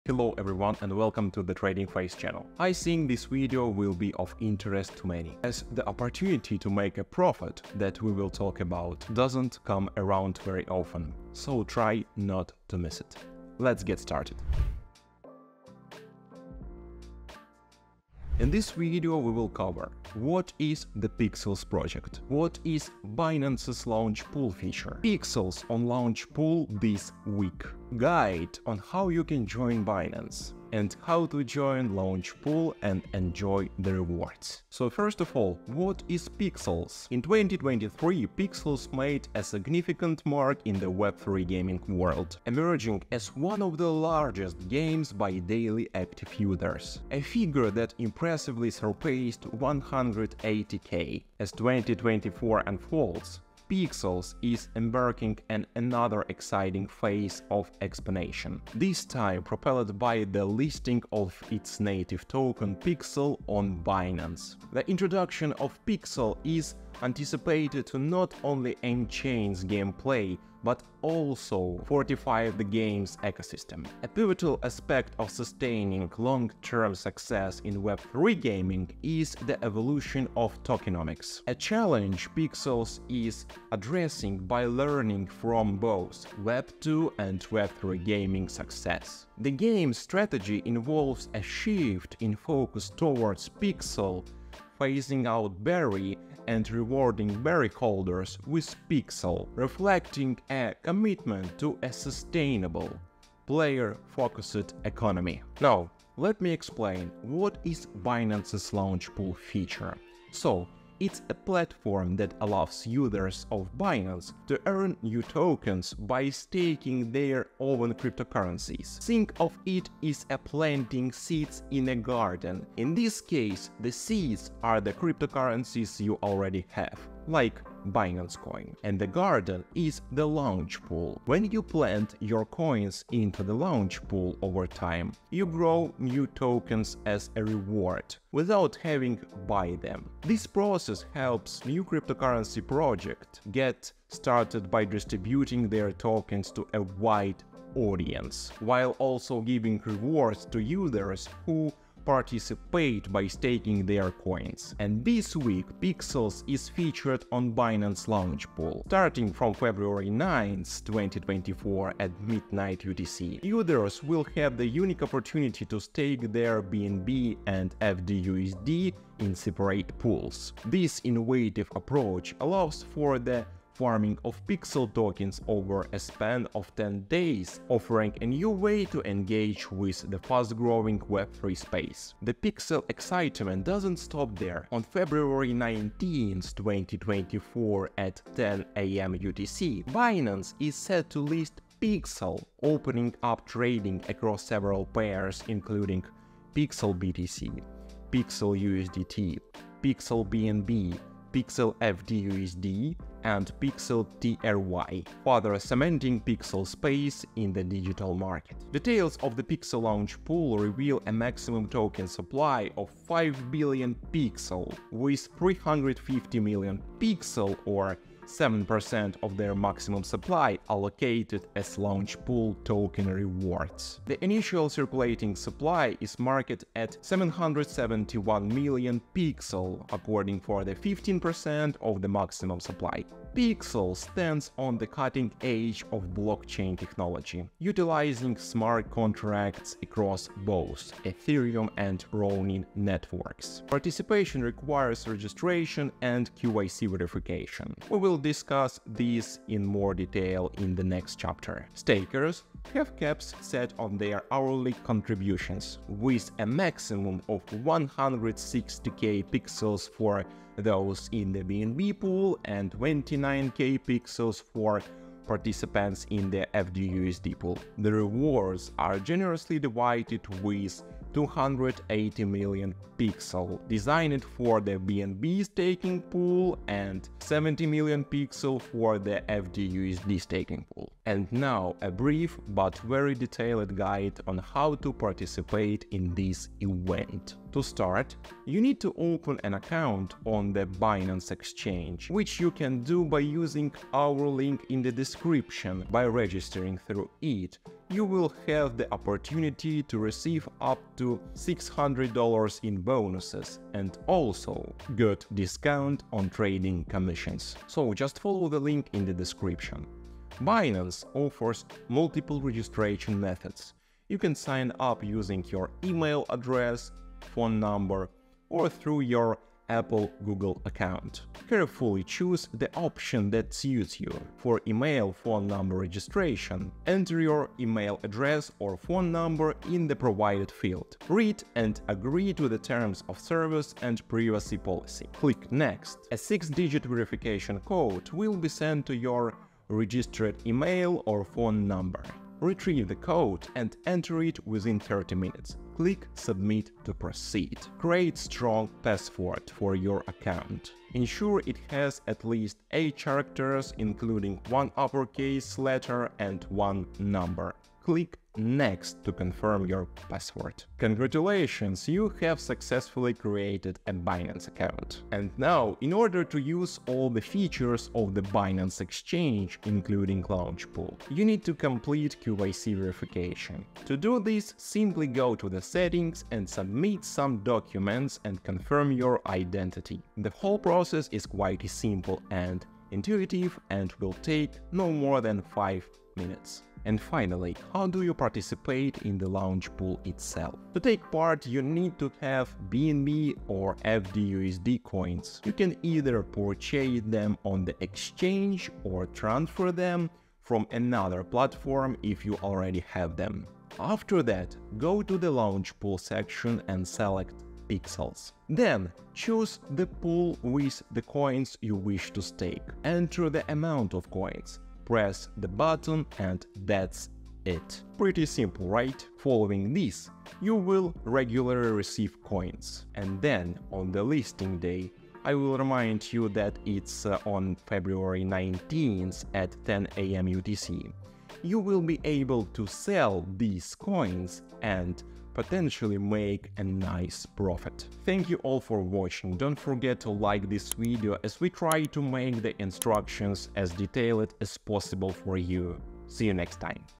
How to use Binance Launchpool/Launchpad (Tutorial)
0:00 - About video and chapters
0:34 - Project PIXELS, explain background of PIXELS and coin
1:45 - What is Binance's Launchpool feature, and how PIXELS is on there this week?
4:41 - How you can join Binance guide?
6:26 - How to to join Launchpool and enjoy the rewards!

In today's video, we will talk about new crypto coin PORTAL, how it will be listed on the Binance exchange, and how to participate in the Binance Launchpool (complete detailed instructions)